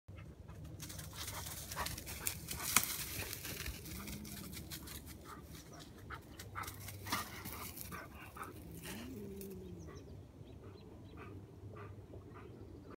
Bernese Mountain Dog Playing with Cat
Here's a quick video of our Bernese Mountain Dog playing with our cat.